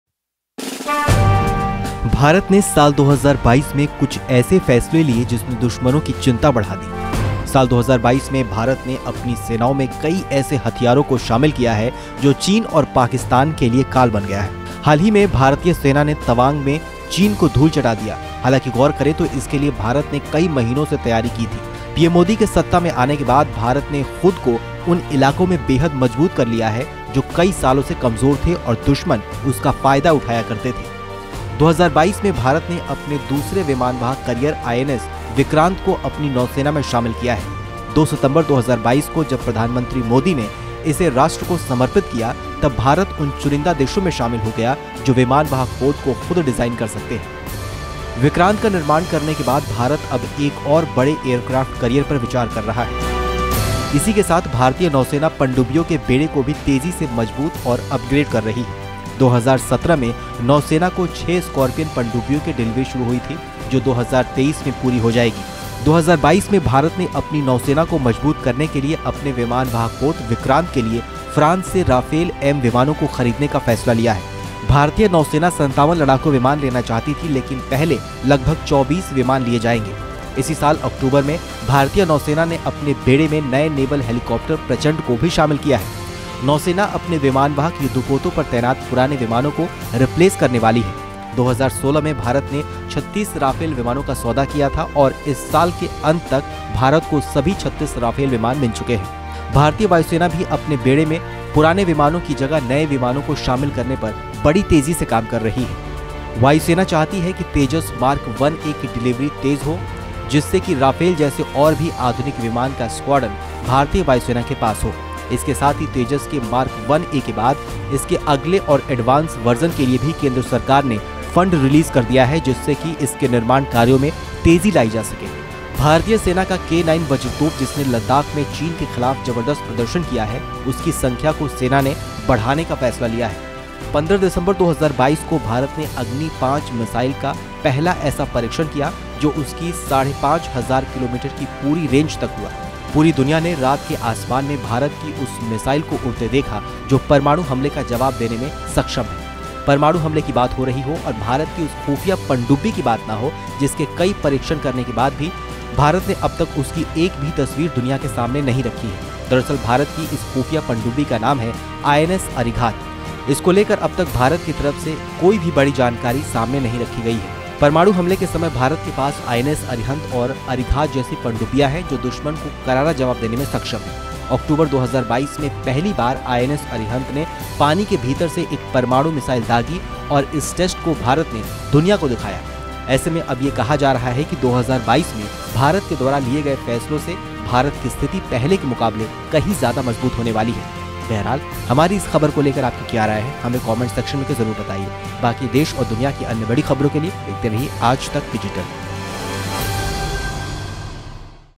2022 में PM Modi ने अपने बड़े फैसलों से दुनिया को चौंकाया, China से Pakistan तक सब हैरान ! |Army
2022 में भारत ने अपने दूसरे विमानवाहक करियर  INS विक्रांत को अपनी नौसेना में शामिल किया। 2 सितंबर 2022 को जब प्रधानमंत्री मोदी ने इसे राष्ट्र को समर्पित किया, तब भारत उन चुनिंदा देशों में शामिल हो गया, जो विमानवाहक पोत को खुद डिज़ाइन कर सकते हैं...विक्रांत का निर्माण करने के बाद भारत अब एक और बड़े एयरक्राफ्ट करियर पर विचार कर रही है. .. इसी के साथ भारतीय नौसेना पंडिब्बियों के बेड़े को भी तेजी से अपग्रेड और मजबूर कर रही है.

In 2022, India inducted its second aircraft carrier INS Vikrant into its Navy. When Prime Minister Modi dedicated it to the nation on 2 September 2022, India became one of the few countries that can design an aircraft carrier itself… After building Vikrant, India is now on another big aircraft career is considering. .. Along with this, the Indian Navy is also rapidly upgrading and strengthening the fleet of Submarine.